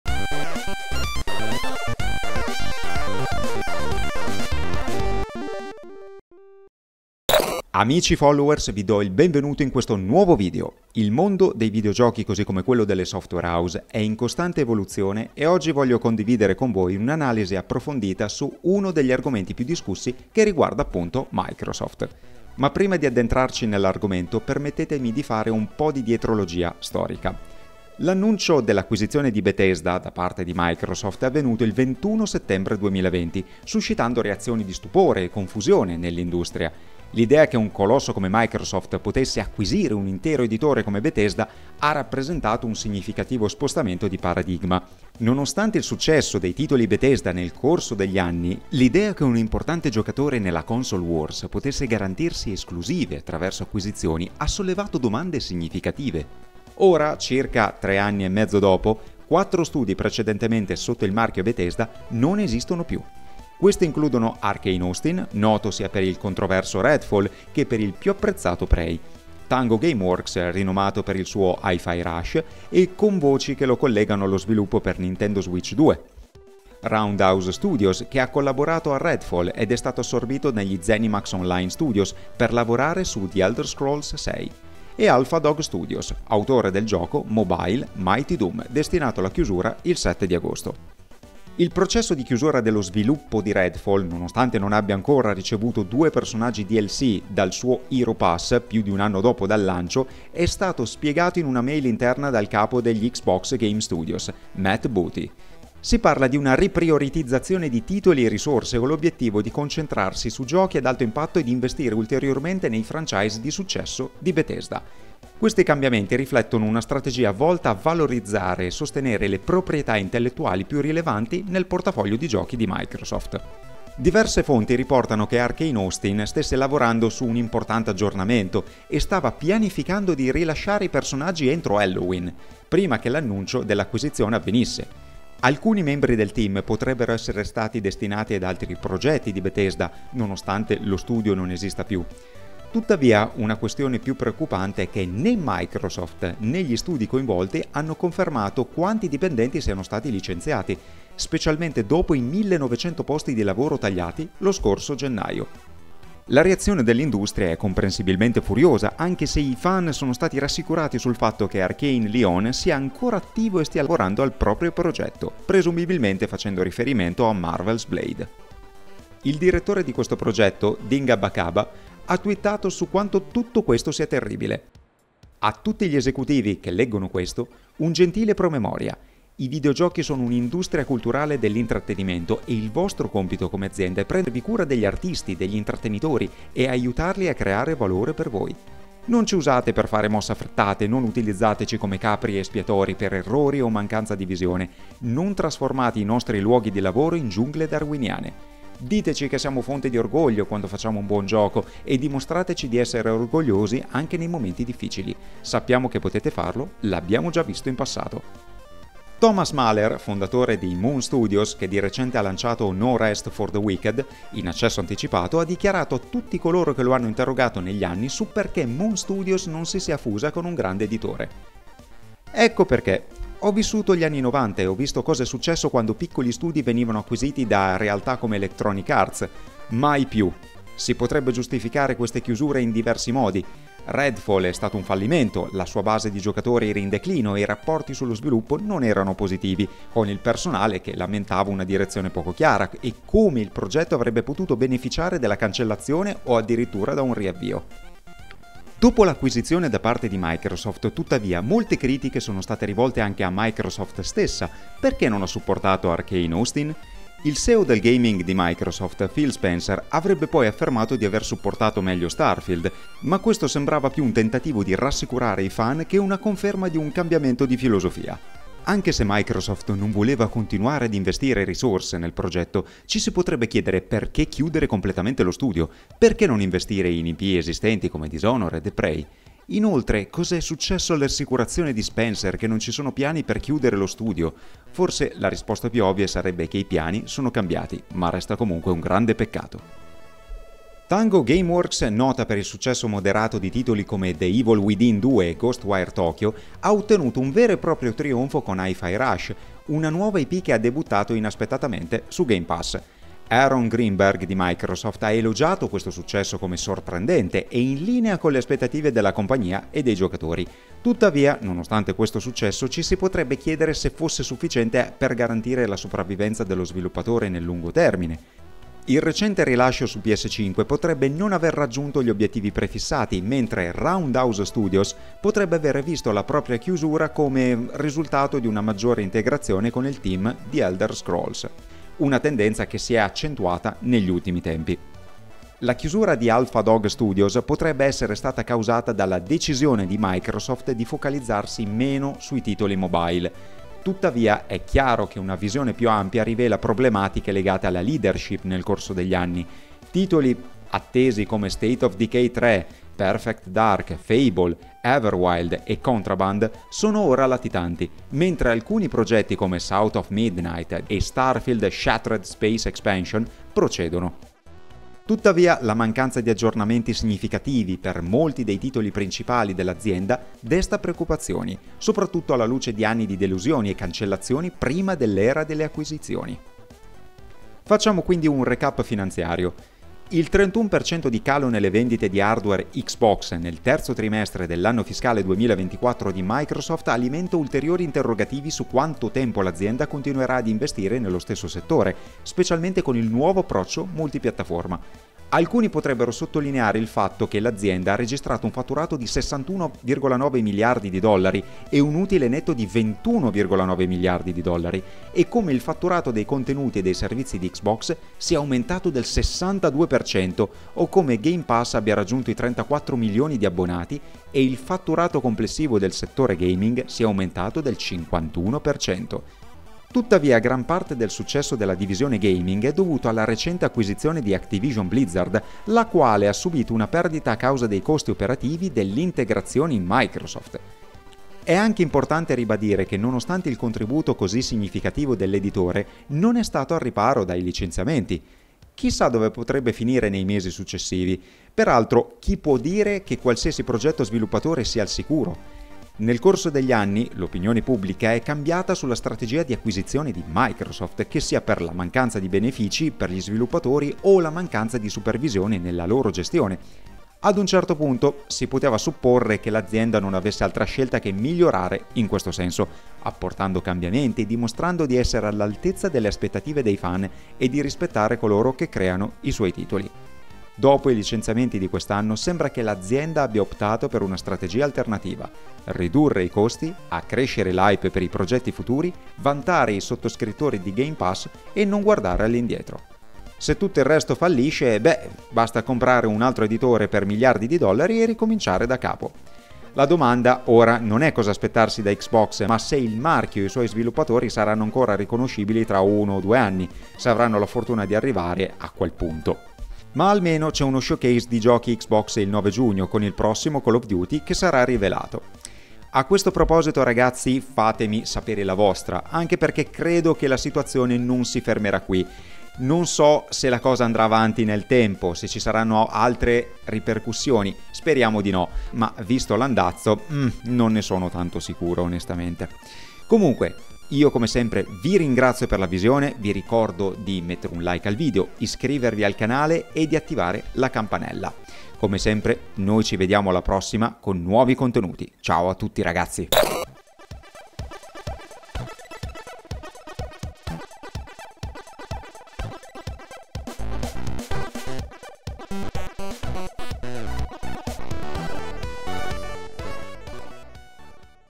XBOX ➤ Microsoft Cosa Combini!?...Ancora!
🕹️ Nel settembre 2020, il mondo dei videogiochi è stato sconvolto dall'annuncio dell'acquisizione di Bethesda da parte di Microsoft. Ma dopo oltre tre anni, cosa è successo ai quattro studi sotto il marchio Bethesda che ora non esistono più? Esploriamo insieme le conseguenze di questa fusione, i progetti interrotti e il destino dei talentuosi sviluppatori coinvolti. Scopriamo come questa mossa ha ridefinito il panorama videoludico e cosa ci riserva il futuro per uno dei più grandi editori di giochi al mondo. 🎮

Info e considerazioni reperite da GamingJolt, The Verge e Twitter.